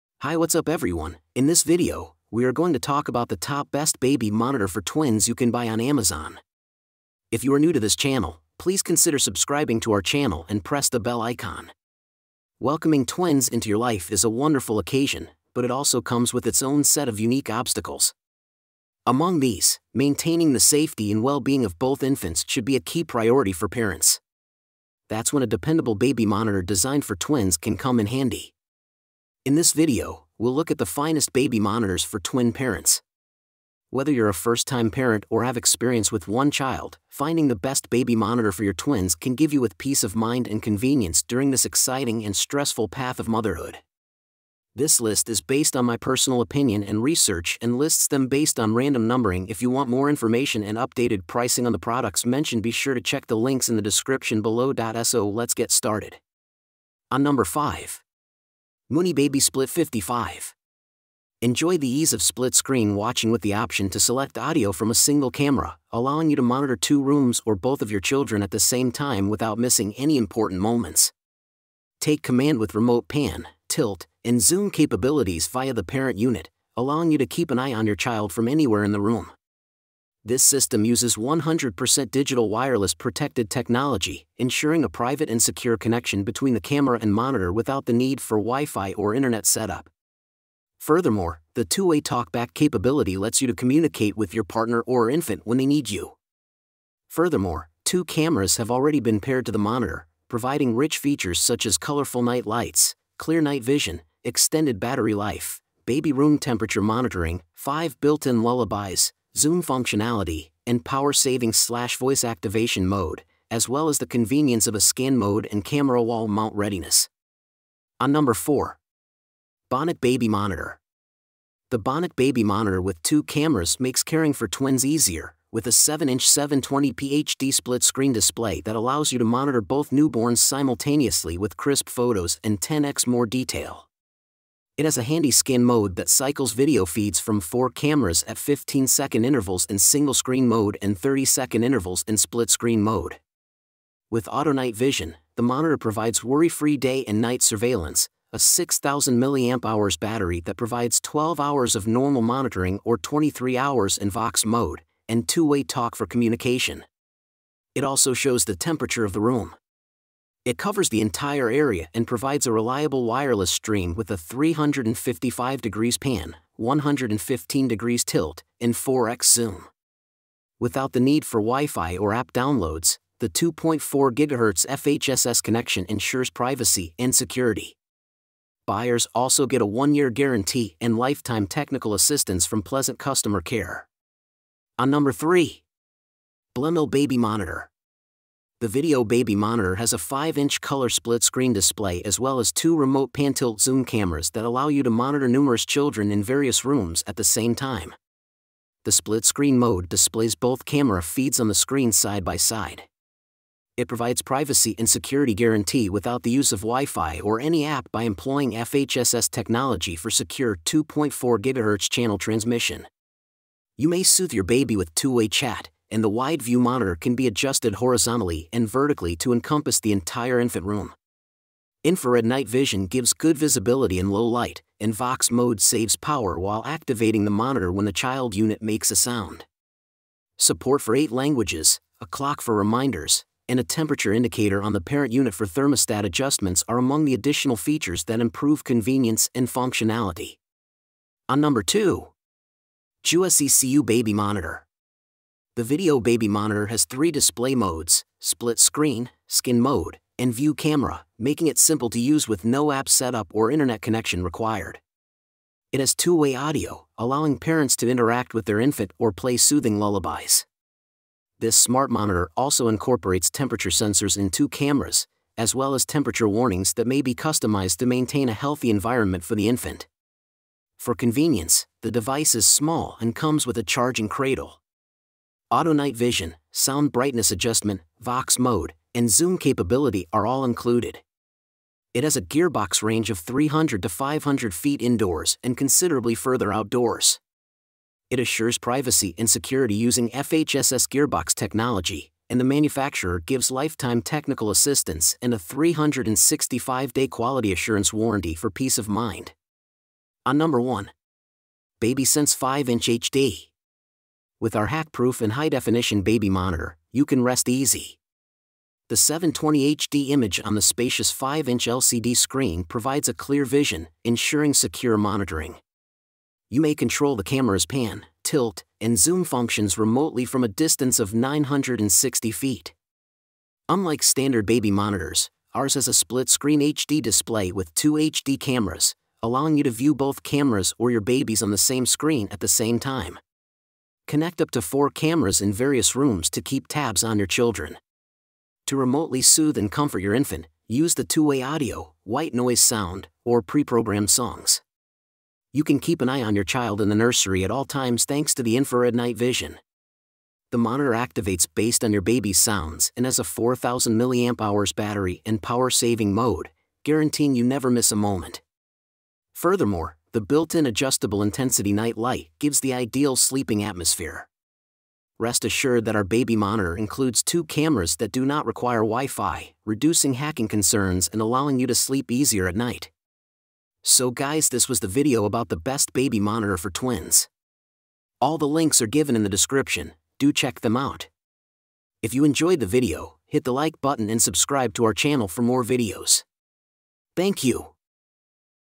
Top 5 Best Baby Monitor For Twins [2025] ✅
Let's talk about the Best Baby Monitor For Twins 2025. I have made a list of Best Baby Monitor For Twins 2025 which will help you decide which one to choose.

Quick Links: ✅✅✅

5: Moonybaby Split 55 0:57
4: bonoch Baby Monitor 1:57
3: Blemil Baby Monitor 3:04
2: JouSecu Baby Monitor 4:01
1: Babysense 5" HD 5:00

I have prepared this video guide to Best Baby Monitor For Twins 2025 to give you the ins and outs to help you make the right choice. So let's get started.